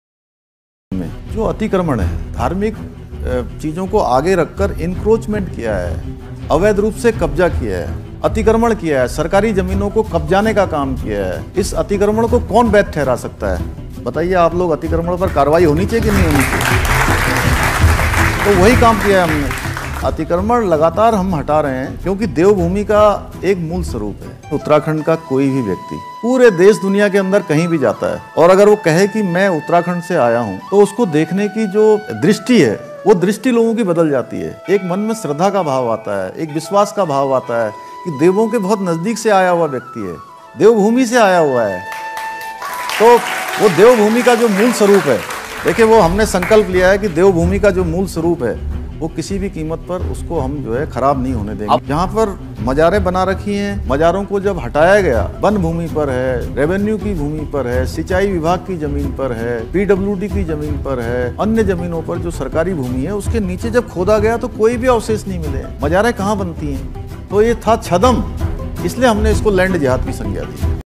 Mazaar Land Jihad ka Hissa hai - CM Pushkar Singh Dhami
Hum Devbhoomi ko wapas uske sahi roop mai lana chahte hai. Log yaha pe Uttarakhand ka demogarphy badalna chahte hai